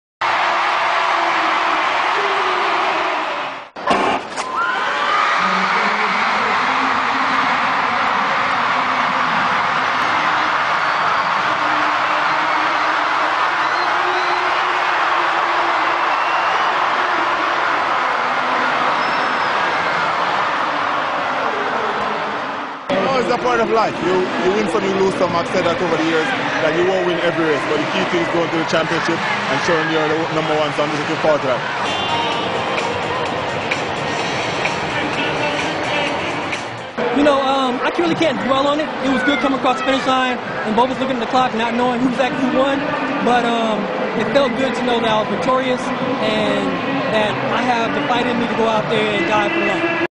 Justin Gatlin Beats Usain Bolt 100m - 9.94 - Golden Gala Rome Diamond League 2013[ORIGINAL]
'Fastest man alive' LOSES 100m sprint: Olympic champion Usain Bolt beaten by American (but at least the race didn't matter)
The Jamaican world record holder, 26, lost to American Justin Gatlin, 31
Former drugs cheat Gatlin finished in 9.94 seconds, Bolt in 9.95 seconds
Bolt shrugged off defeat, saying 'You can't win every race - I've learnt that'
He is still world record holder after running 100m in 9.58 seconds in 2009